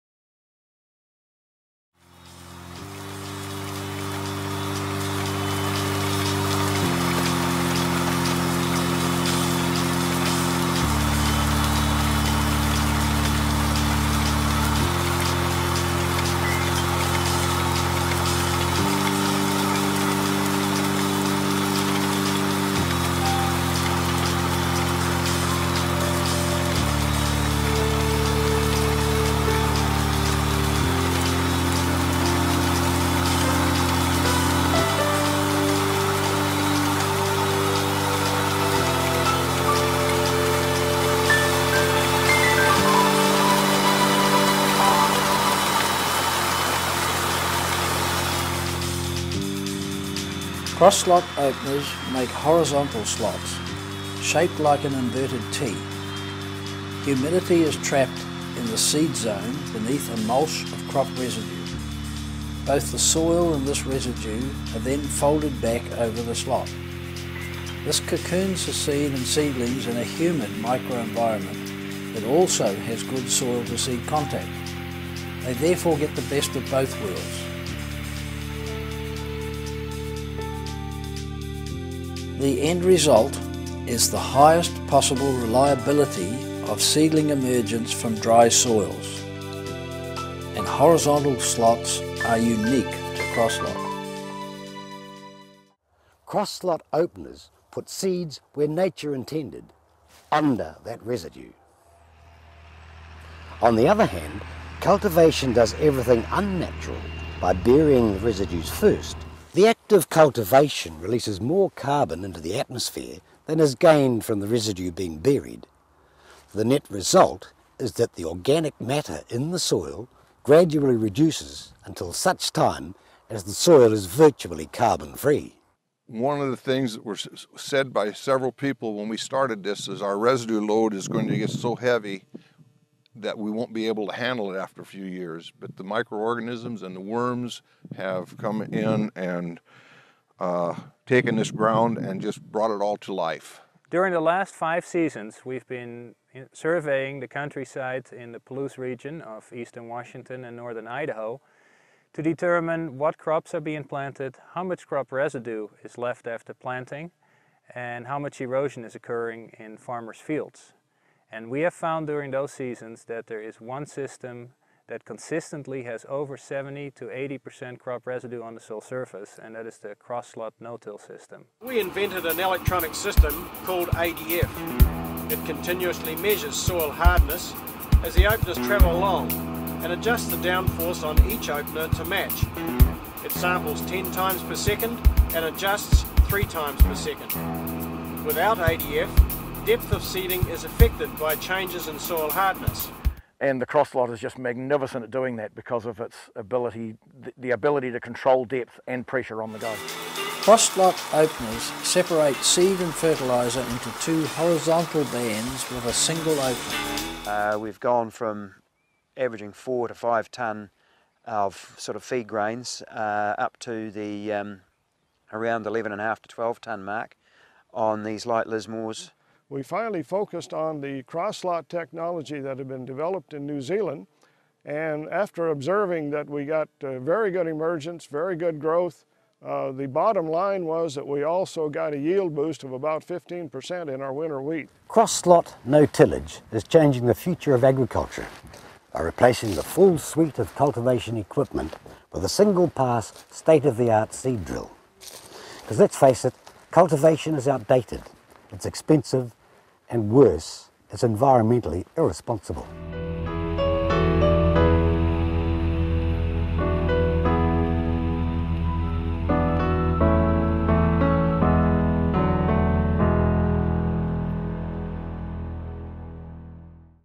Cross Slot TM whats different to other Seeders
Cross Slot TM was a finalist in the Environment category (corporate) at the 2010 WTA in New York. This is the video clip shown at the award dinner . Graphics show how seed and fert are banded thru one opener below surface residue, and shows an overview of the savings and benifits to the environment.Direct seeding without hair pining of straw.